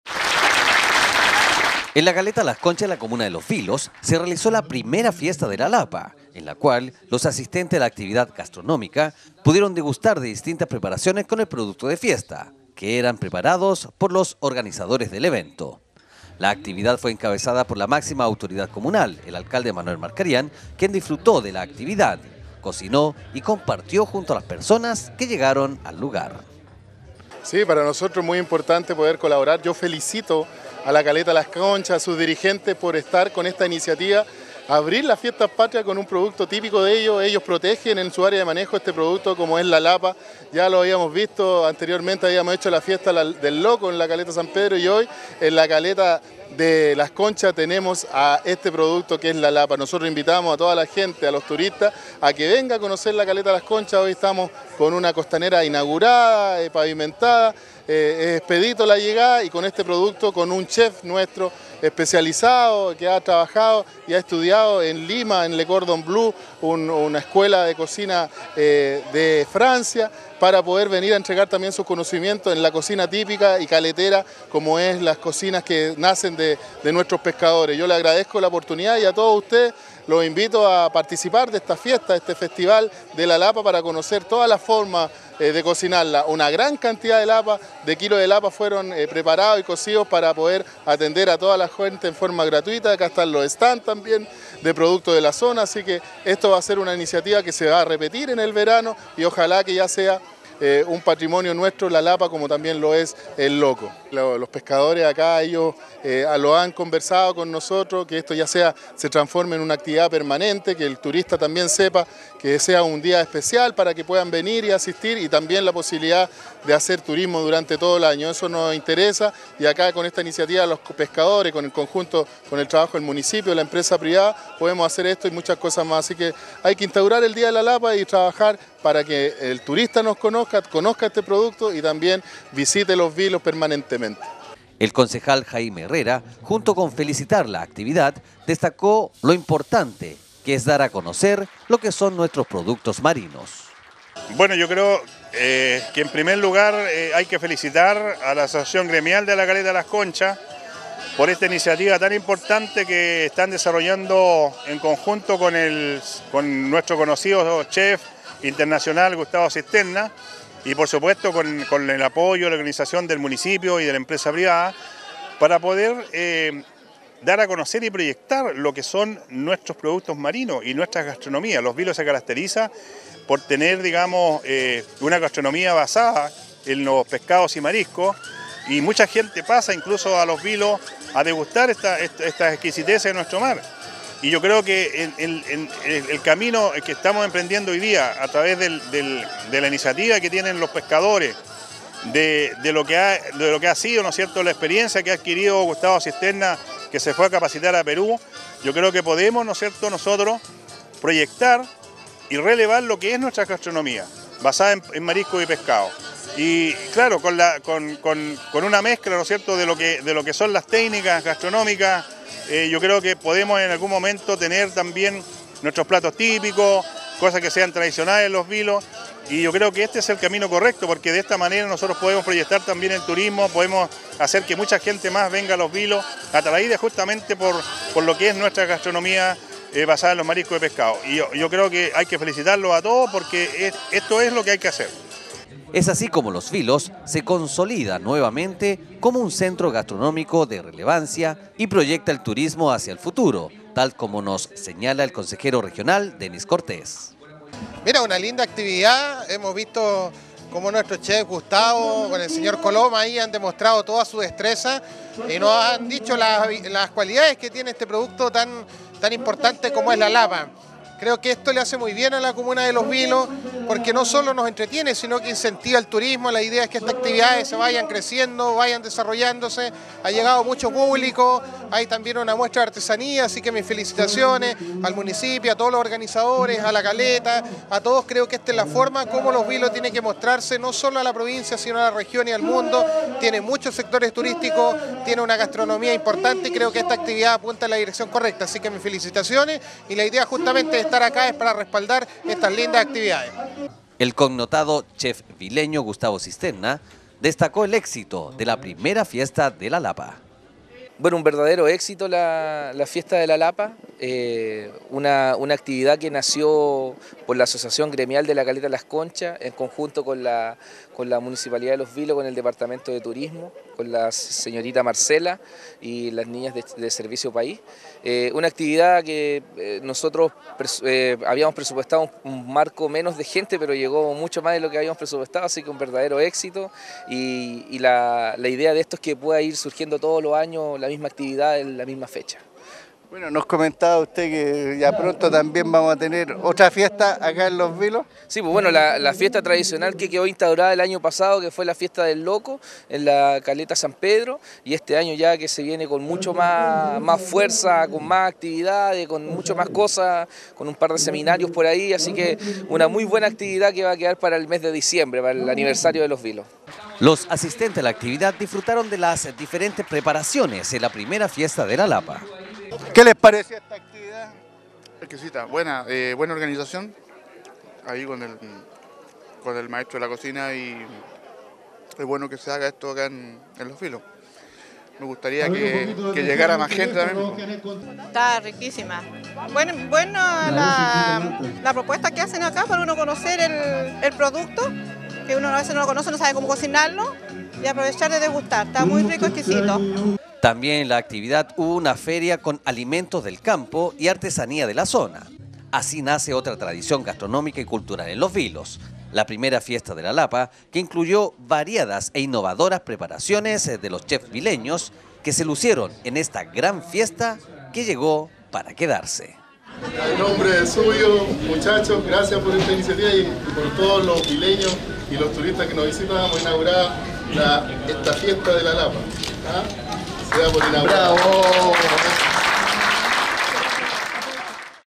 Gran Fiesta de la Lapa en la Caleta Las Conchas.
En la  caleta las Conchas de la comuna de Los Vilos se realizó la primera la Fiesta de la Lapa, en la cual los asistentes a la actividad gastronómica, pudieron degustar distintas preparaciones con el producto de fiesta  que eran preparados por los organizadores del evento.
La actividad  fue encabezada por la máxima autoridad comunal, el alcalde Manuel Marcarian quien disfrutó de la actividad, cocinó y  compartió junto a las personas que llegaron al lugar.
El concejal Jaime Herrera junto con felicitar la actividad, destacó que lo importante es dar a conocer lo que son nuestros productos marinos.
Es así como los Vilos se consolida nuevamente como un centro gastronómico de relevancia y proyecta el turismo hacia el futuro, tal como nos señala el consejero regional Denis Cortés.
El connotado chef vileño   Gustavo Cisternas destacó el éxito de la primera Fiesta de la Lapa.
Los asistentes a la actividad disfrutaron de las preparaciones en la primera fiesta de la lapa.
También en la actividad hubo una feria con alimentos del campo y artesanías de la zona. Así nace otra tradición gastronómica y cultural en Los Vilos, la primera Fiesta de la Lapa que incluyó variadas e innovadoras preparaciones de los chefs Vileños que se lucieron en esta gran fiesta que llegó para quedarse.